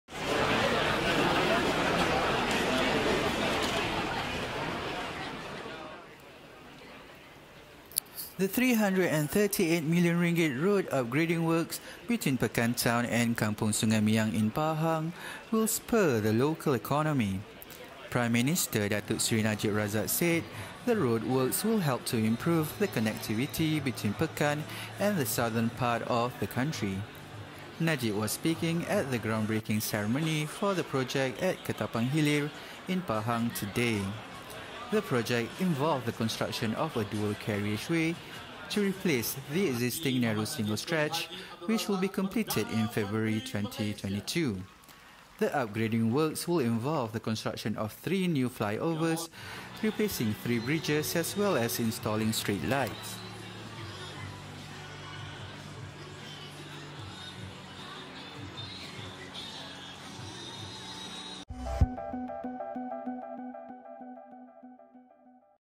PM launch works to upgrade trunk road in Pekan
Upon completion, the project, part of the Highway Network Development Plan(HNDP) from Pekan to Kota Tinggi, Johor is expected to boost the farming, fisheries, tourism and industrial activities along the stretch.